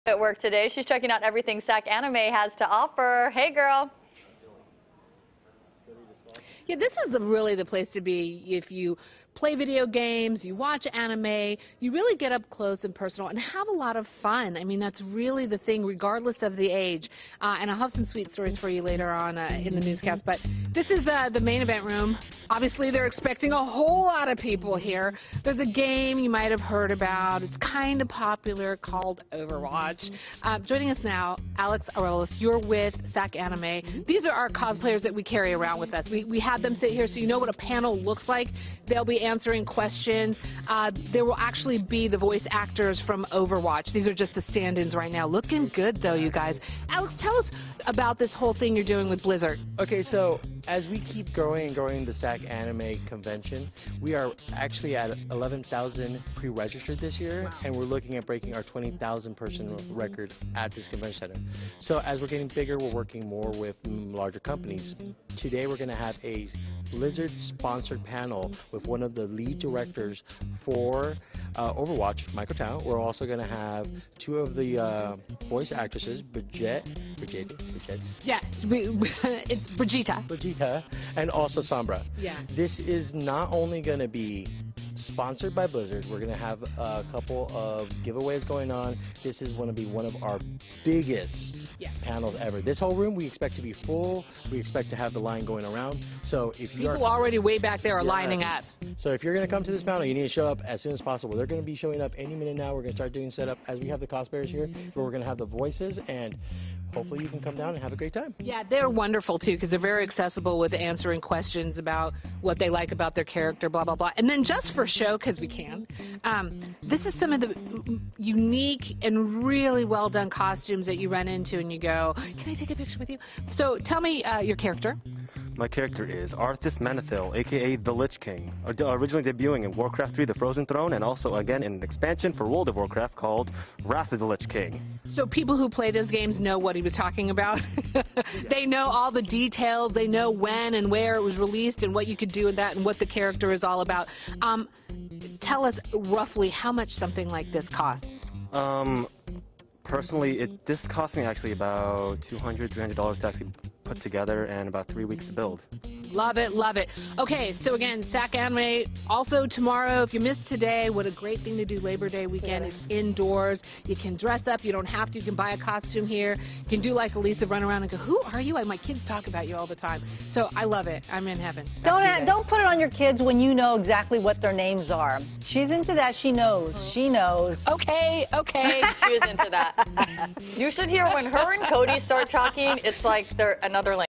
SacAnime Summer 2018 Pt. 3
It’s the convention for all things pop culture and cosplay it’s SacAnime Weekend! Asisa Becerra is talking with the attendants!